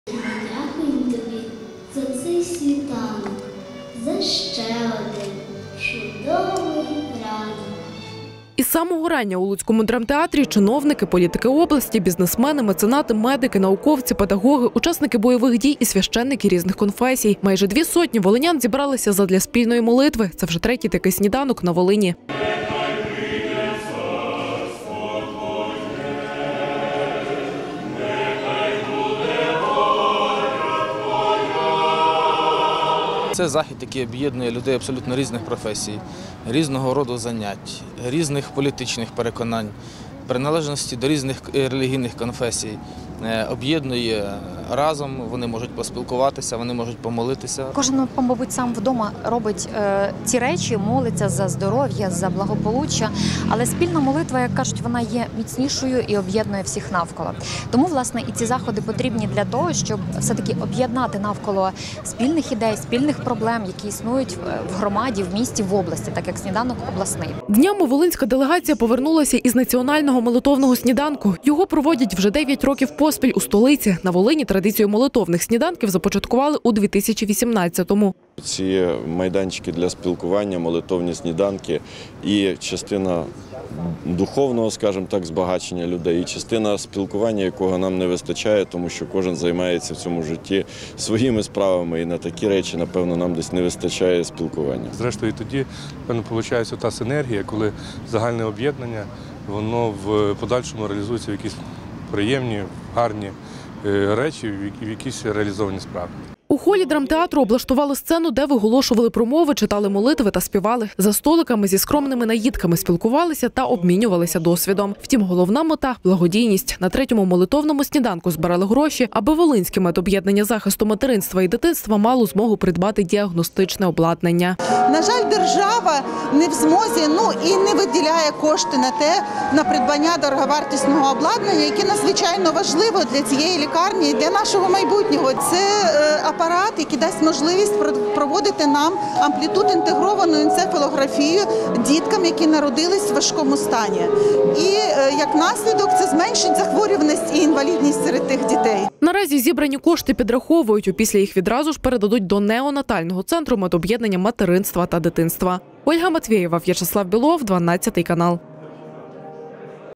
Яким був Третій молитовний сніданок Волині
На Волині на Третьому молитивному сніданку зібралися осадовці, бізнесмени, меценати, священники, аби разом помолитися за область та зібрати гроші для неонатального центру Волинського медоб'єднання.

«Ці майданчики для спілкування, молитовні сніданки – і частина духовного збагачення людей, і частина спілкування, якого нам не вистачає, тому що кожен займається своїми справами і на таку речі нам десь трохи не вистачає спілкування», – каже народний депутат України Вячеслав Рубльов.

Утім, головна мета заходу – благодійність. На Третьому молитовному сніданку збирали гроші, аби Волинське медоб'єднання захисту материнства і дитинства мало змогу придбати діагностичне обладнання.

Детальніше – у сюжеті 12 каналу.